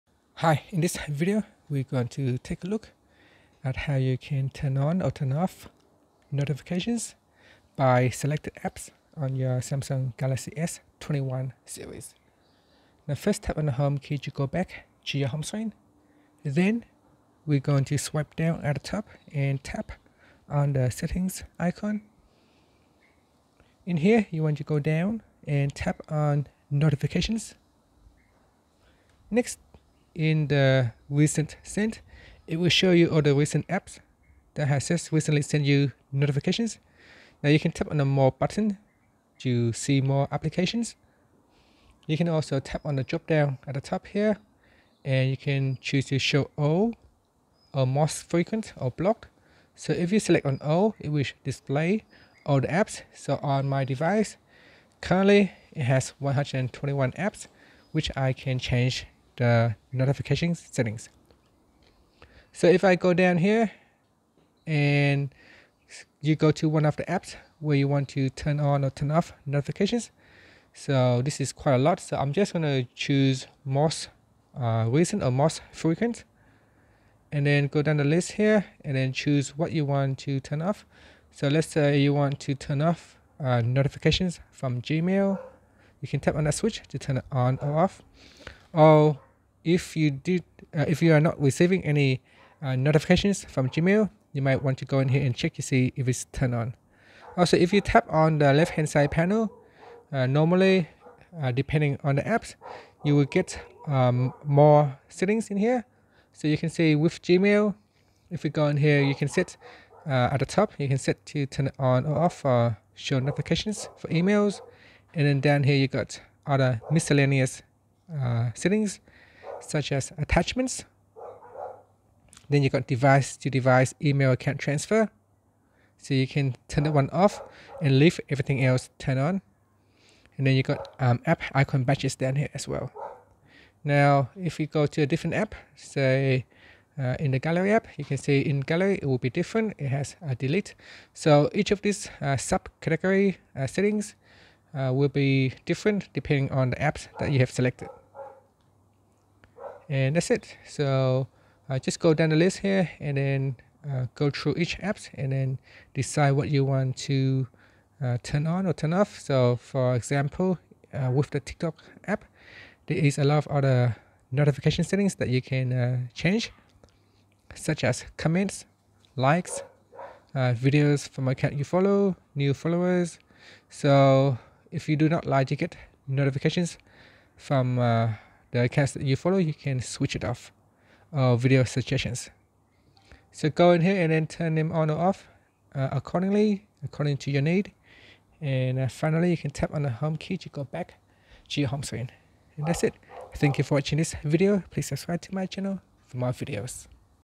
Galaxy S21/Ultra/Plus: How to Turn On/Off Notifications By Selected Apps
Learn how you can turn on or off notifications by selected apps on the Galaxy S21/Ultra/Plus.